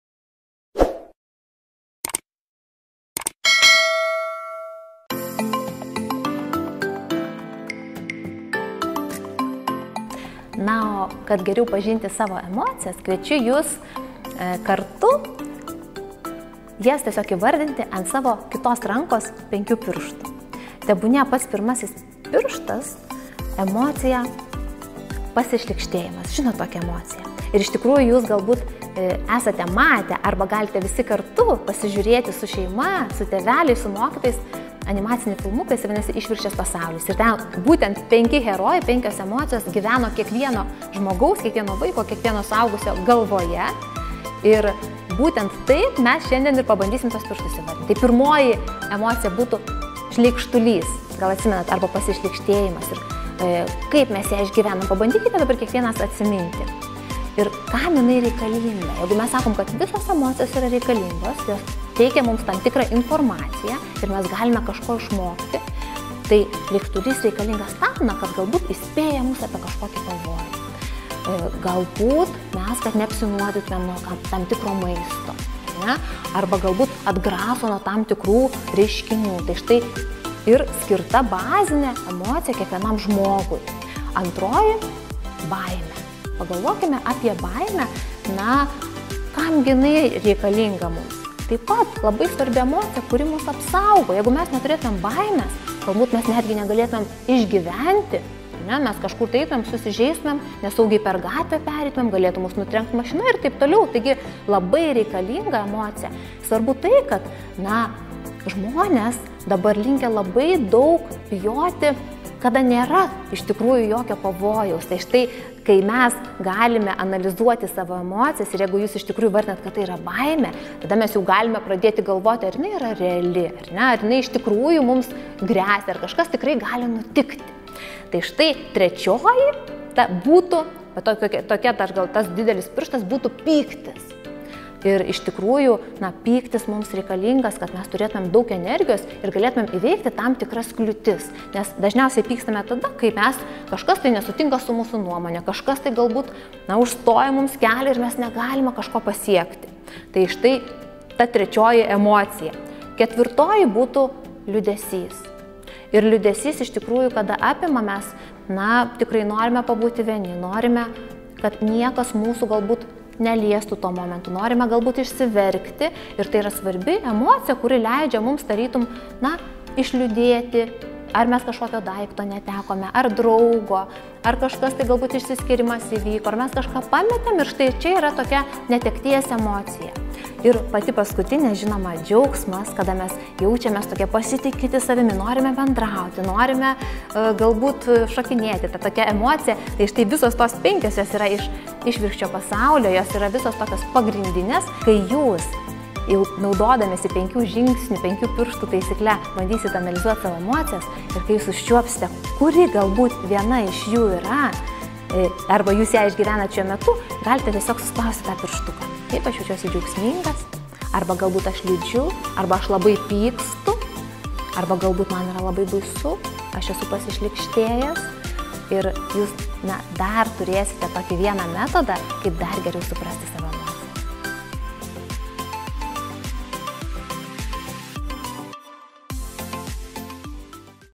Olimpinis mėnuo 2020 | Emocinė sveikata | 5
Trečioji "Olimpinio mėnesio" savaitė skirta emocinei sveikatai. Video reportaže - vaikams apie dar vieną metodą emocijų atpažinimui ir valdymui.

Tūkstančiai pradinukų jau išbandė įvairias mankštas, šoko, žaidė aktyvius žaidimus, išgirdo daug apie sveiką mitybą ir formavo naujus įgūdžius!